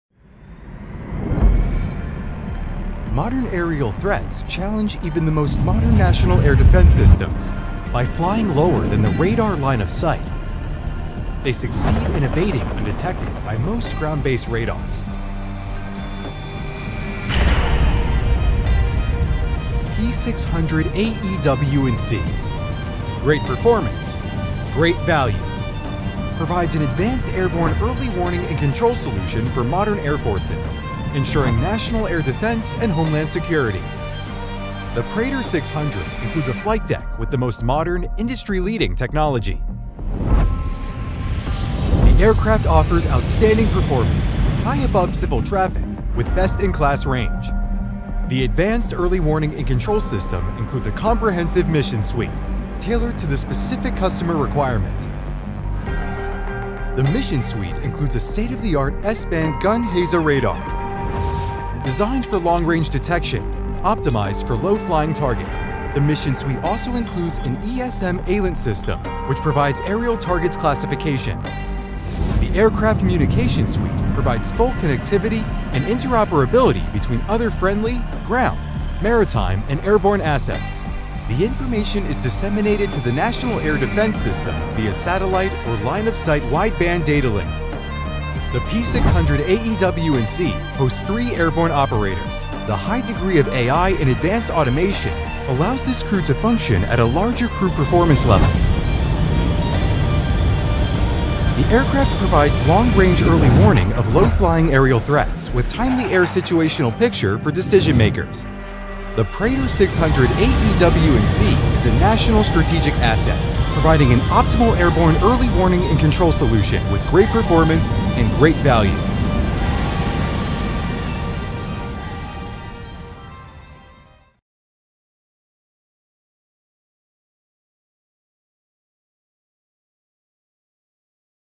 IAI ELTA and EMBRAER P600 AEW Airborne Early Warning
P600 AEW (Airborne Early Warning) is a next generation aircraft based on the advanced super midsize platform of the Embraer Praetor 600 business jet. The P600 AEW can provide an extended Air Situational Picture by monitoring aerial activity in areas outside ground radar coverage. It can perform various missions such as Air Defense, Early Warning, Command and Control, Fighter Fleet Efficiency, Territorial Defense, and Maritime Surveillance.The P600 AEW can be configured with the full range of AEW&C sensor and control systems: Digital AESA AEW Radar, civil and military IFF, ESM/ELINT with Radar Warning Receiver capability, Command & Control, comprehensive communication suite including Data Networks and Satellite Links, and a robust Self Protection Suite (SPS).

The P600 AEW addresses the growing market of Intelligence, Surveillance and Reconnaissance capabilities for countries that require cost-effective, high performance and flexible solutions for defense and homeland security missions. The Embraer Praetor 600 is the best performing super-midsize bizjet in its category, offering intercontinental range with an outstanding payload capability, high availability and reliability, short turn-around and low life cycle costs. Coupled with ELTA’s superior radar technology, the P600 AEW offers the benefits of cutting edge, proven systems and provides capabilities available until now only on much larger platforms.